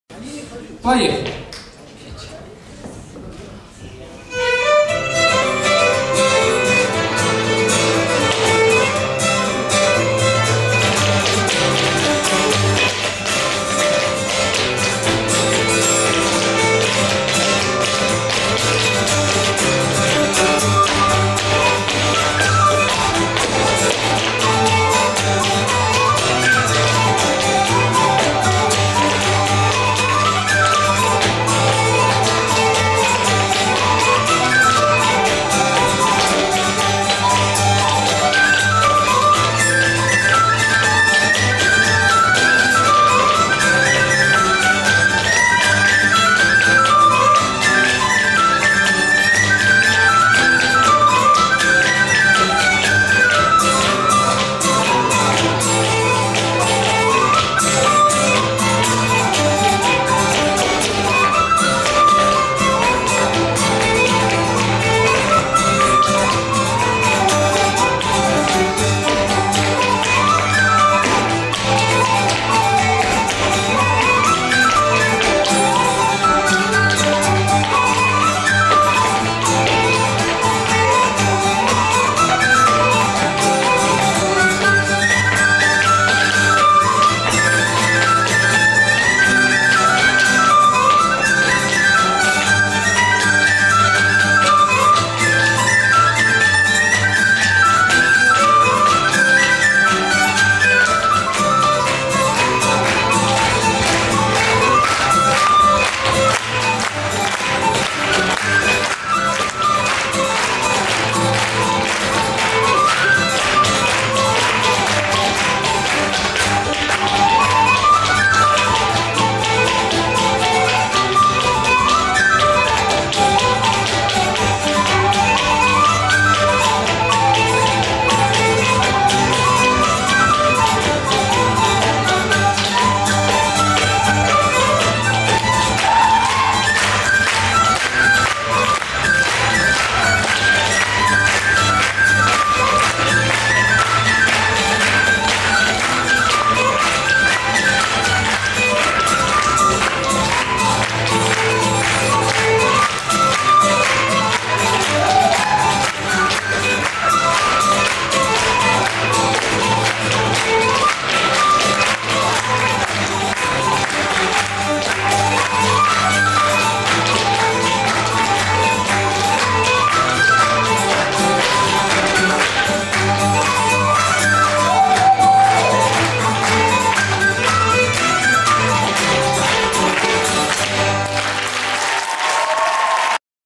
First attempt to brush-dance
Samhain Fest 2010, Moscow, Russia
Dancing by Natalia Malysheva and Alexey Popov, music by Slua Si band (Moscow)

There's a little joke based on a brush dance. We did it for the first time and I hope we didn't mess it up. Not completely anyway :)